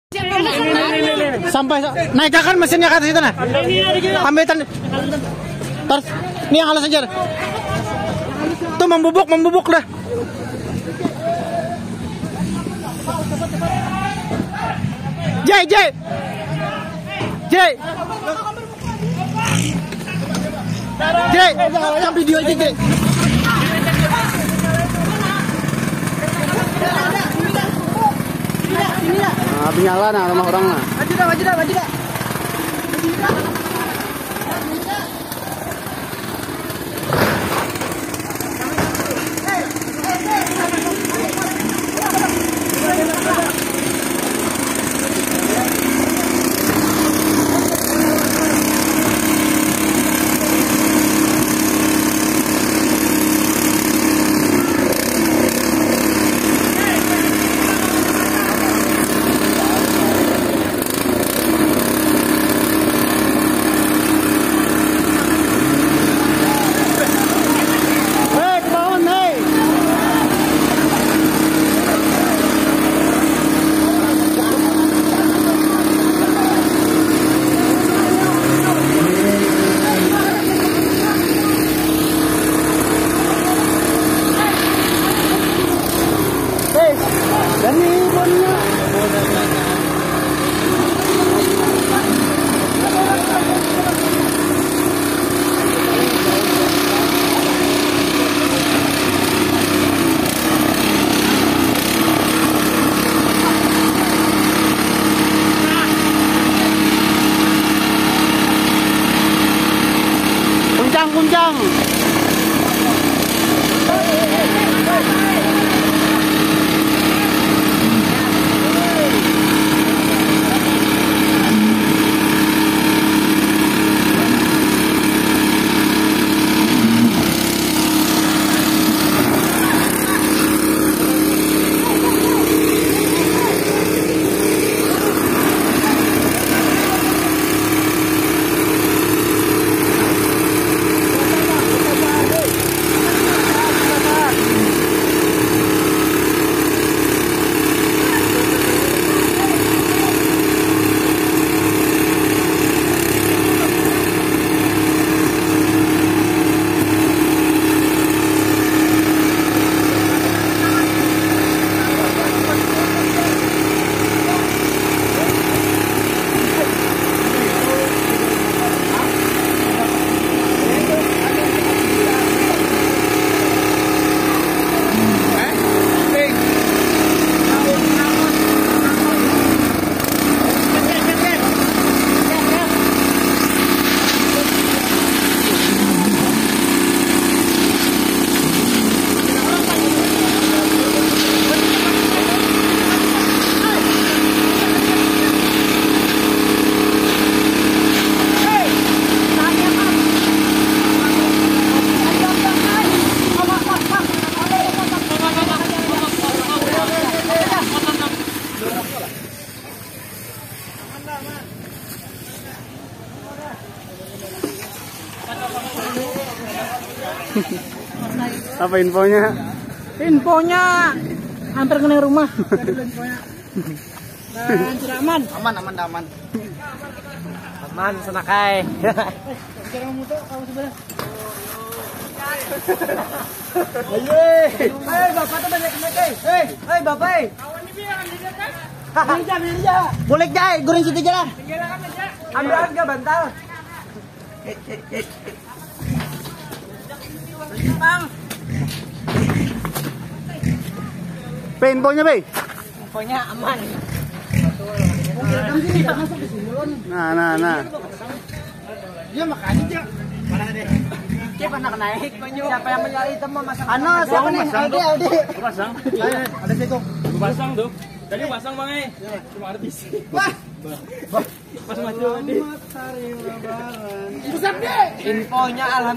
677# respon cepat jaya 65 TKM kebakaran a yani km 17 kab banjar september 2023
677# respon cepat jaya 65 TKM kebakaran a yani km 17 kab banjar september 2023

selalu support channel ini dan juga channel gaming dari saya